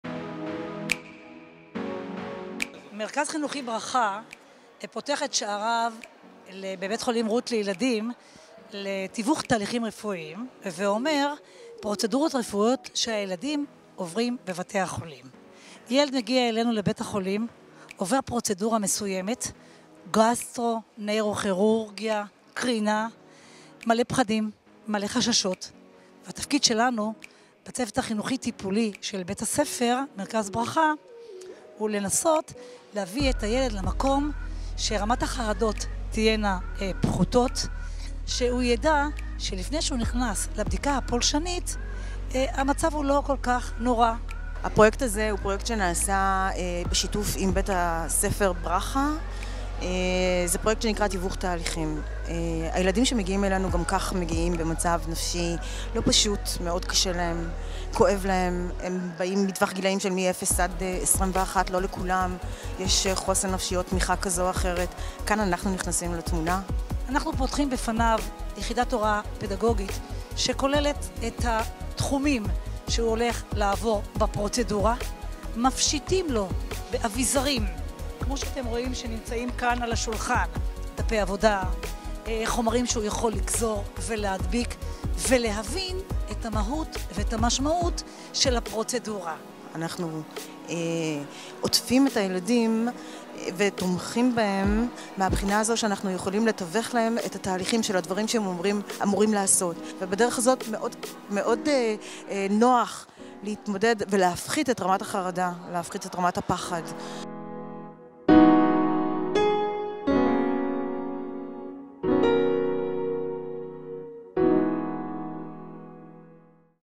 כשבובות ולגו הופכים לכלים בהפחתת כאב בילדים
ביקור בבית החולים הוא לרוב חוויה לא נעימה ואפילו קצת מפחידה, במיוחד כשמדובר בילדים. כחלק מתפיסת "בית חולים ללא כאב" אותו מוביל בית החולים "רות" רפפורט לילדים שברמב"ם, הפכו בשנים האחרונות עזרי הסברה "לא רגילים" לחלק בלתי נפרד מההכנה של המטופלים הקטנטנים להליכים רפואיים שונים. לפני כמה ימים אירח ביה"ח "רות" יום עיון בנושא, כשצוות מרכז חינוכי ברכ"ה, המפעיל את כיתות הלימוד בביה"ח, העביר את הידע והניסיון שצבר בתחום לגורמים בתחום החינוך מכל רחבי הארץ.

עצמות ידיים ממקלות אוזניים, מכשיר MRI מקוביות לגו ומסע אל המעיים באמצעות רובוט – כך מלמדים את הילדים המגיעים לטיפול ברמב"ם על התהליכים הרפואיים שהם עתידים לעבור ומפחיתים אצלם את רמות החרדה.

תאריך 26/02/2020